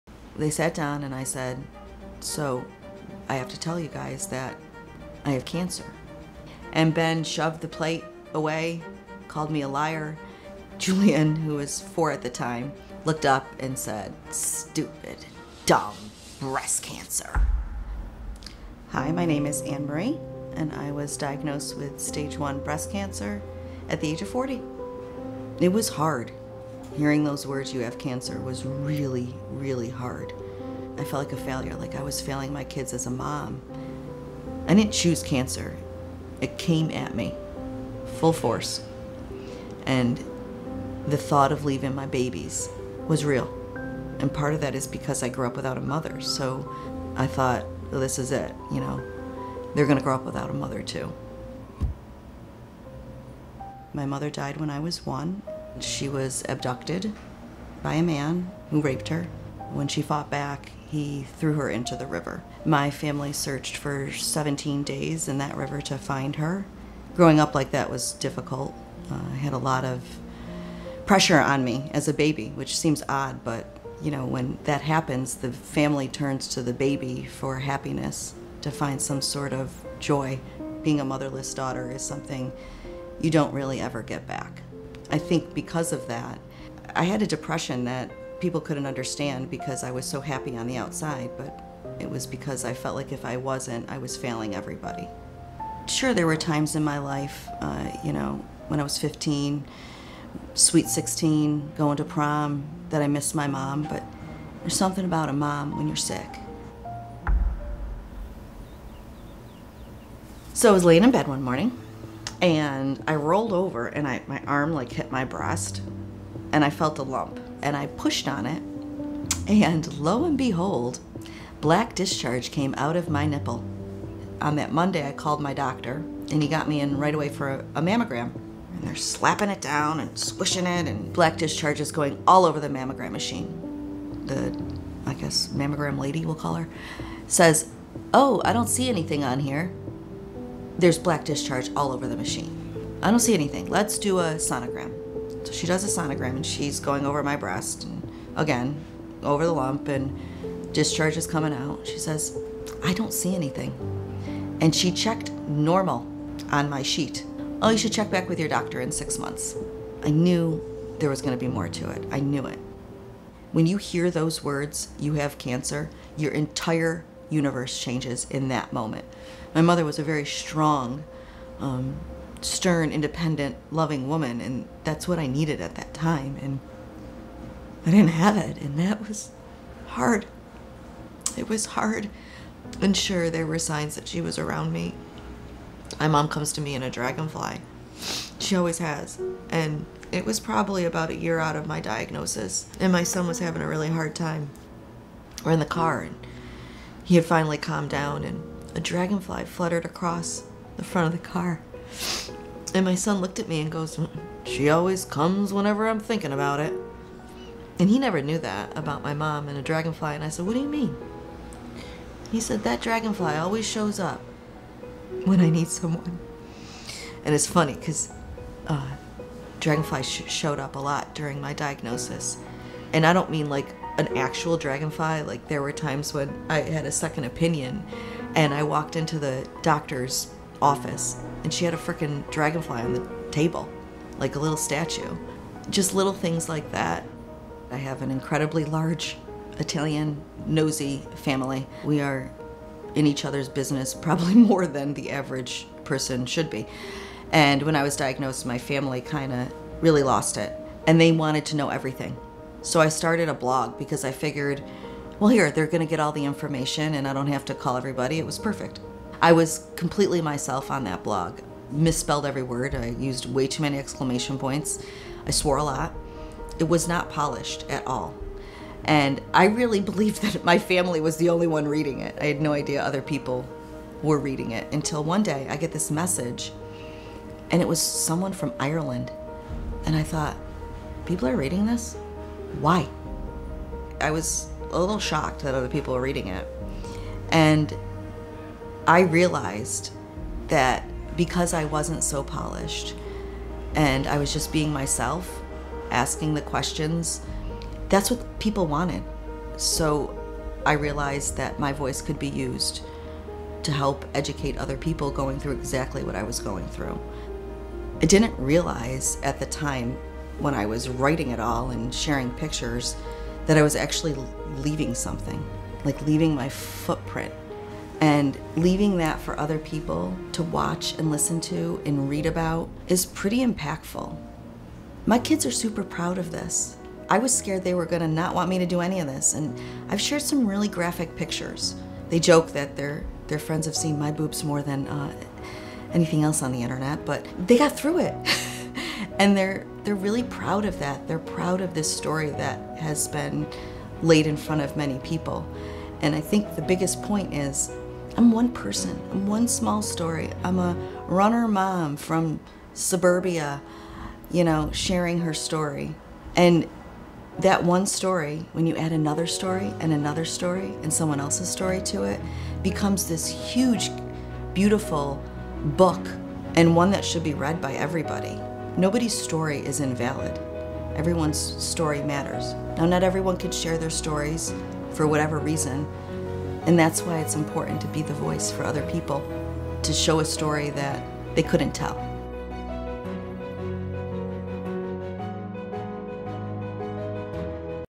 Stupid Dumb Breast Cancer
I thought, “This is it.” “They’re going to grow up without a Mother, too.” Meet Annmarie. She accidentally revolutionized breast cancer advocacy through a personal blog intended for family and friends and that is when Stupid Dumb Breast Cancer came to life. (Link in comments). Now, she is helping people around the world in advocacy with help from her sons and one special dragonfly.

This is Annmarie’s legacy.
What’s yours?